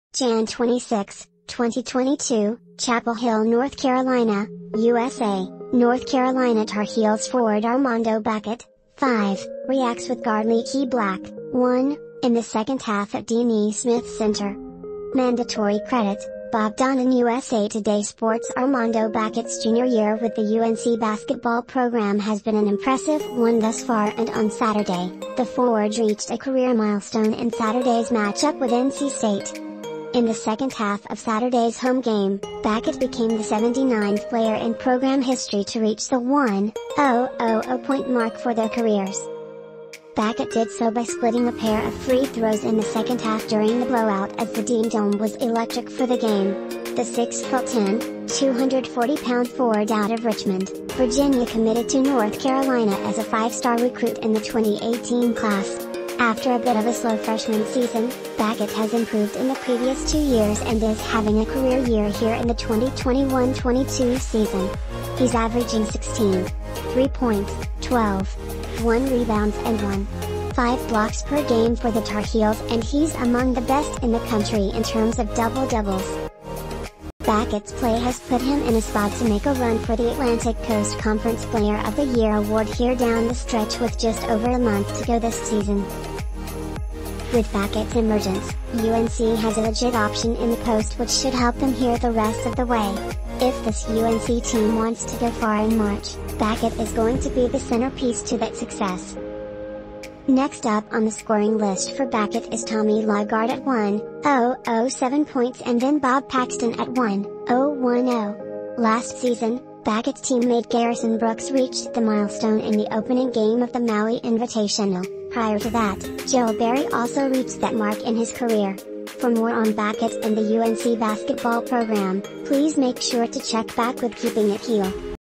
UNC Basketball Armando Bacot reaches career milestone
Jan 26, 2022; Chapel Hill, North Carolina, USA; North Carolina Tar Heels forward Armando Bacot (5) reacts with guard Leaky Black (1) in the second half at Dean E. Smith Center. Mandatory Credit: Bob Donnan-USA TODAY SportsArmando Bacot’s junior year with the UNC basketball program has been an impressive one thus far and on Saturday, the forward reached a career milestone in Saturday’s matchup with NC State. In the second half of Saturday’s home game, Bacot became the 79th player in program history to reach the 1,000-point mark for their careers. Bacot did so by splitting a pair of free throws in the second half during the blowout as the Dean Dome was electric for the game. The 6-foot-10, 240-pound forward out of Richmond, Virginia committed to North Carolina as a five-star recruit in the 2018 class. After a bit of a slow freshman season, Bacot has improved in the previous two years and is having a career year here in the 2021-22 season. He’s averaging 16.3 points, 12.1 rebounds and 1.5 blocks per game for the Tar Heels and he’s among the best in the country in terms of double-doubles. Bacot’s play has put him in a spot to make a run for the Atlantic Coast Conference Player of the Year award here down the stretch with just over a month to go this season. With Bacot’s emergence, UNC has a legit option in the post which should help them here the rest of the way. If this UNC team wants to go far in March, Bacot is going to be the center piece to that success. Next up on the scoring list for Bacot is Tommy LaGarde at 1,007 points and then Bob Paxton at 1,010. Last season, Bacot’s teammate Garrison Brooks reached the milestone in the opening game of the Maui Invitational. Prior to that, Joel Berry also reached that mark in his career. For more on Bacot and the UNC Basketball program, please make sure to check back with Keeping it Heel.